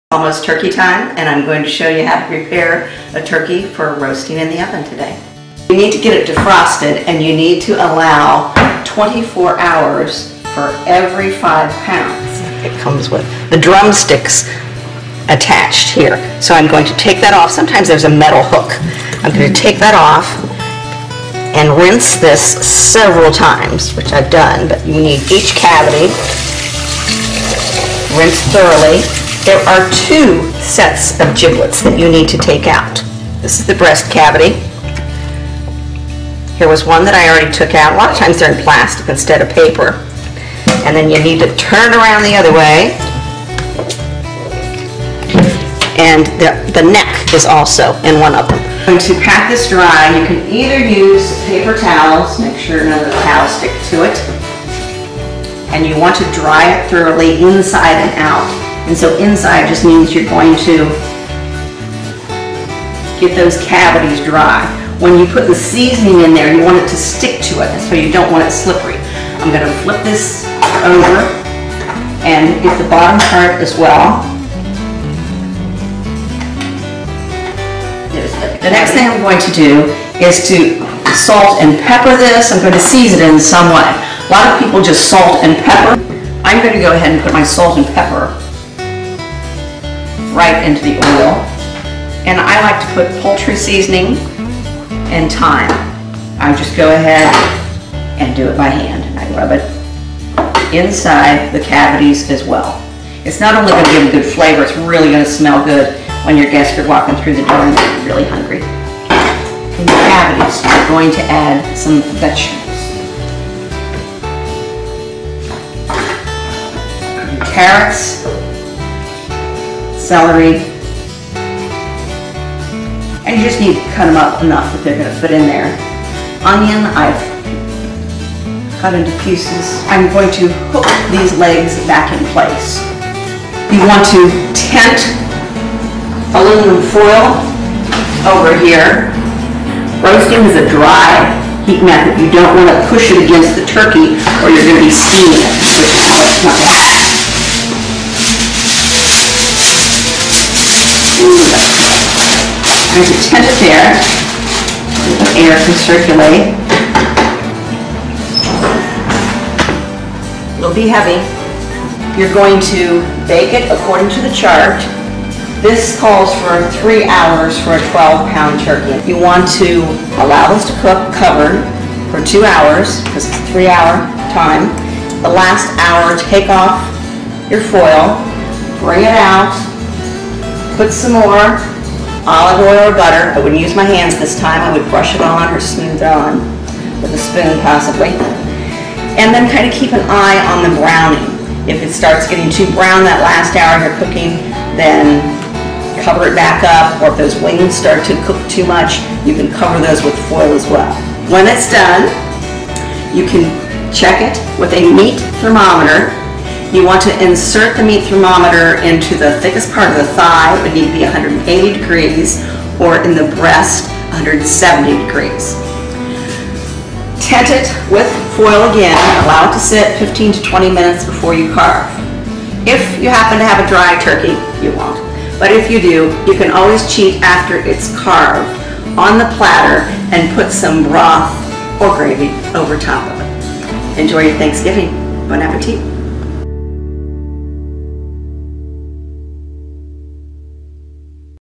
Turkey Fun - Roasting a Turkey in the Oven
Learn how to prepare a turkey for roasting in the oven for Thanksgiving.